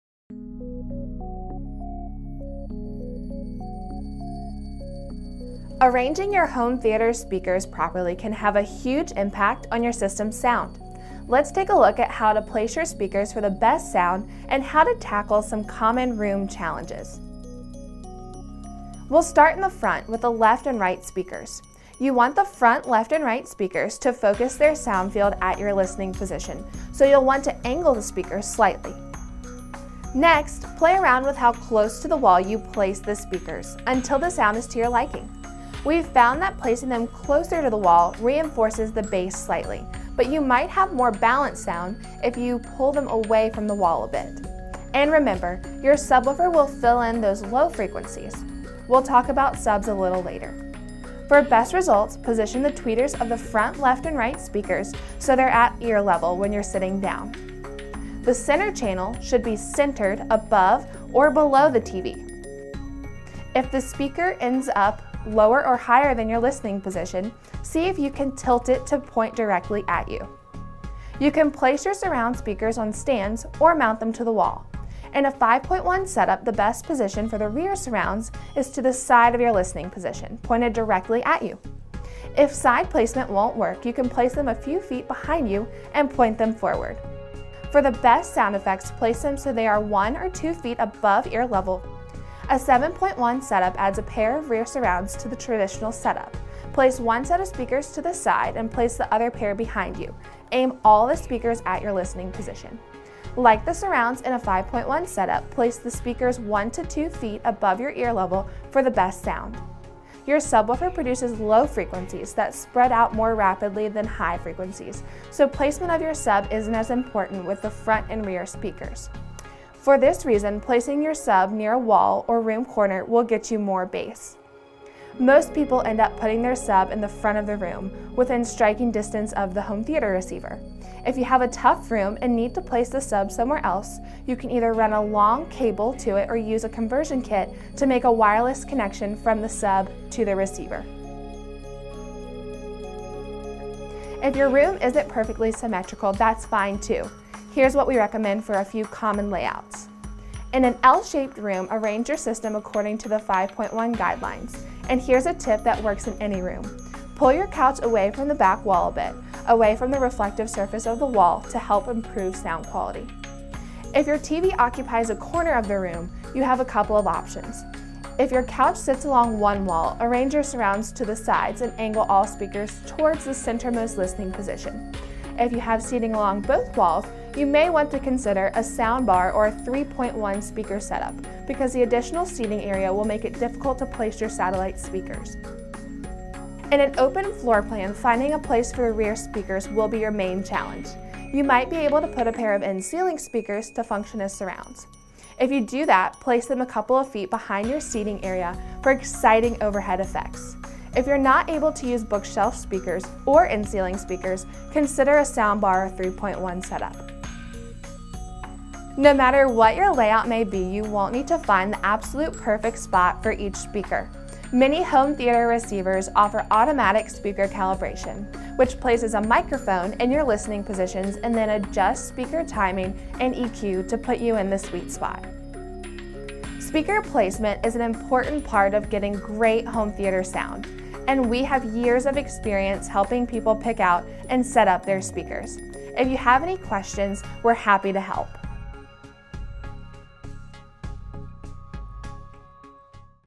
Where to place speakers in your home theater | Crutchfield video
Placement of your home theater speakers can make a huge difference in your sound. In this video, we'll show you some tips that'll help you get the best sound out of your setup.